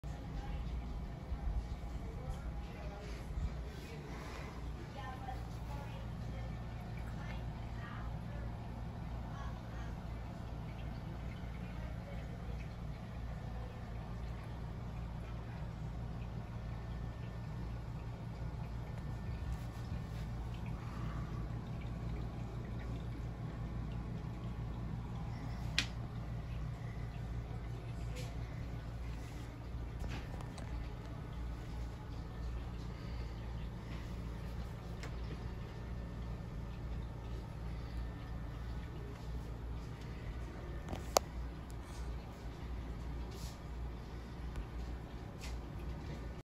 Diamondback terrapin clown loach fish tank
6 foot long tank

Oldest clown loach is 11 years old

Mix of ages